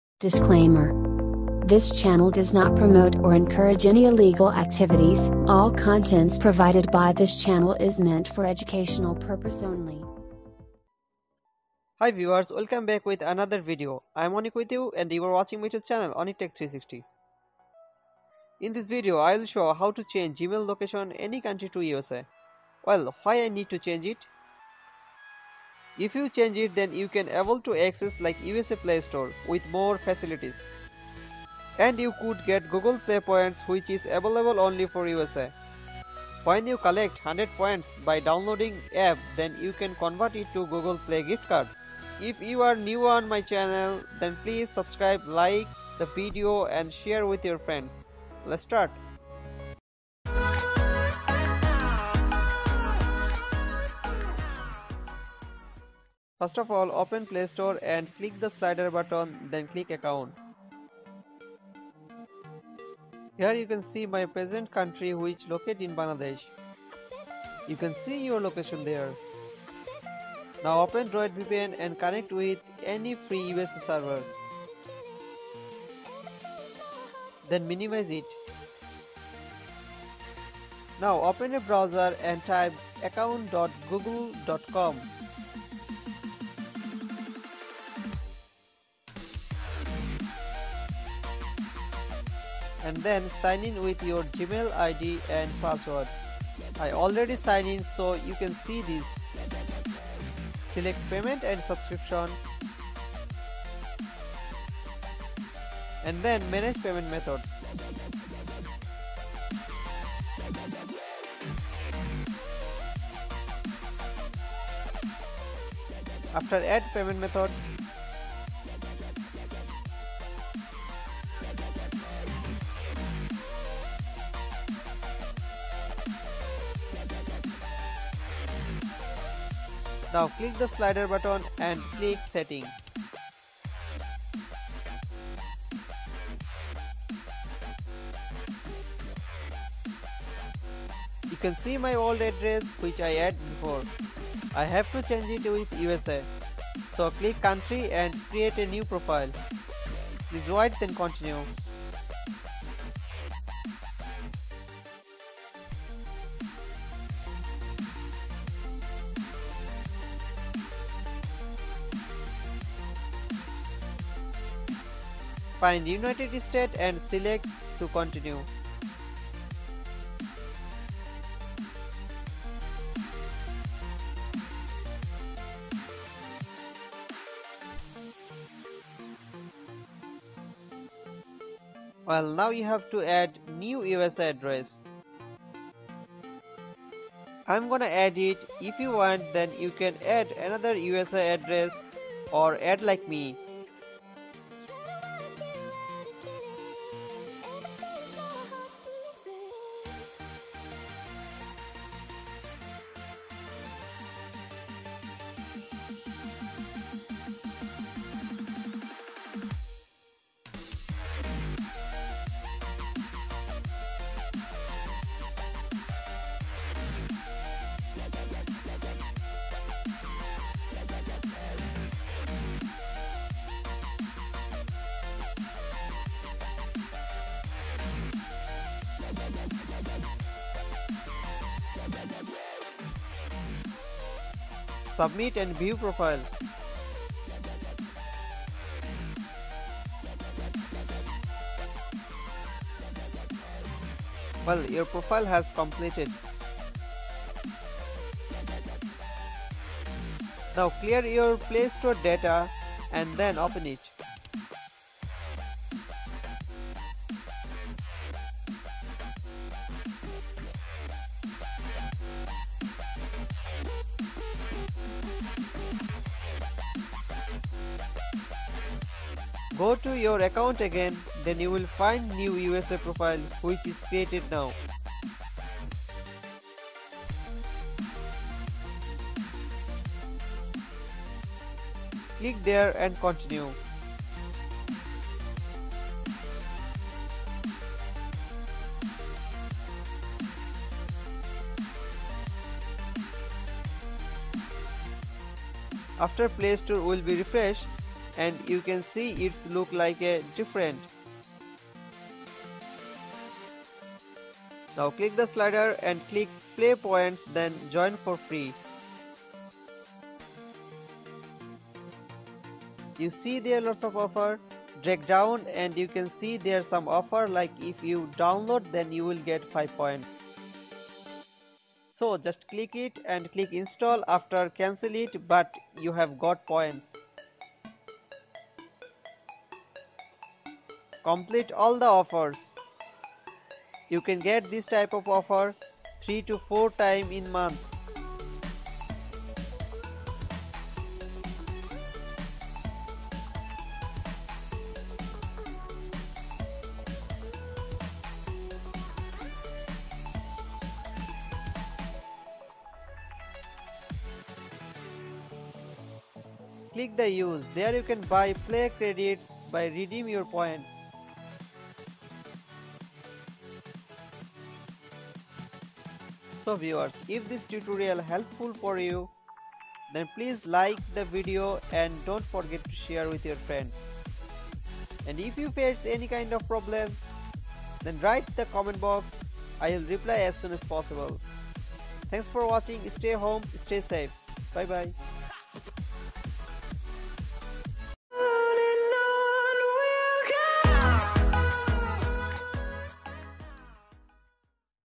change country in playstore any country to usa
Hi viewers if you want to change Gmail account location Any country to USA then please watch this video full-time . Then you'll able to get free Google play points.

Download brave browser earn Free money
link 👇 👇👇 👇